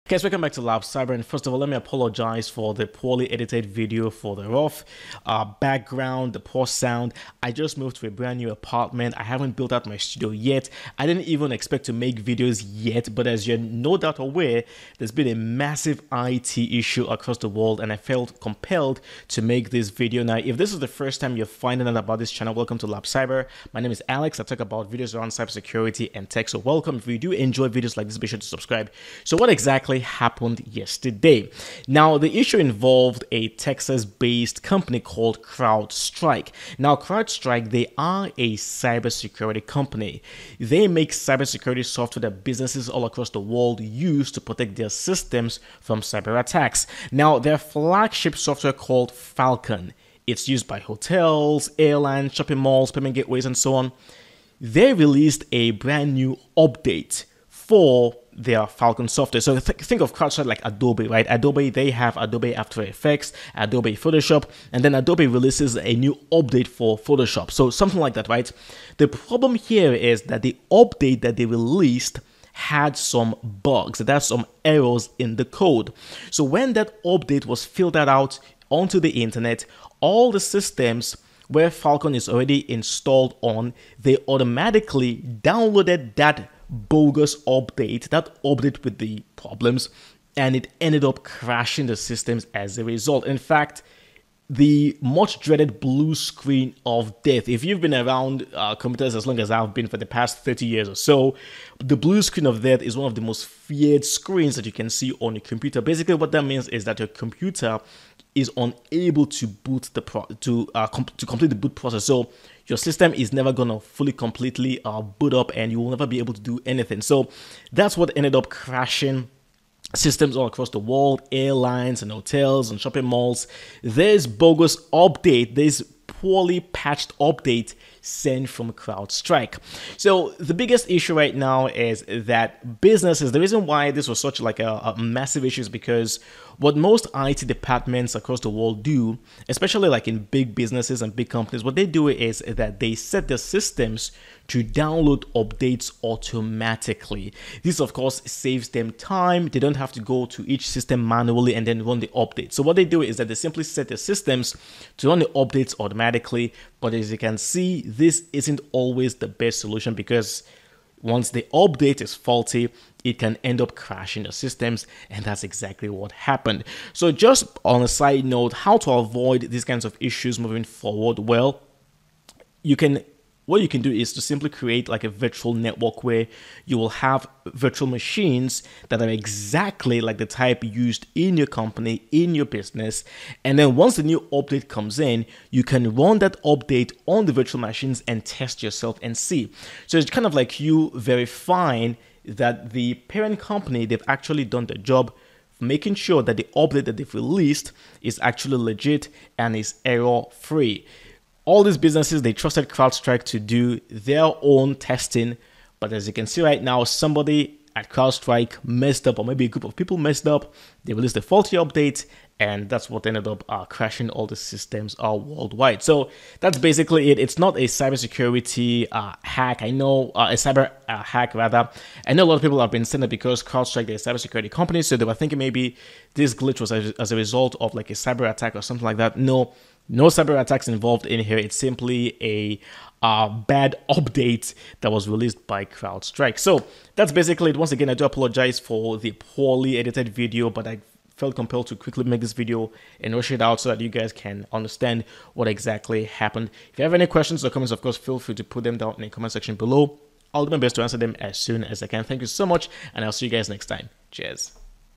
The Biggest IT Outage Ever Explained - CrowdStrike Strikes Bad !!!
Yesterday the biggest IT outage occurred when tens of thousands of computers all over the world started to crash. In this video, I will explain to you exactly what happened.

Would you like to enroll in my cyber security courses?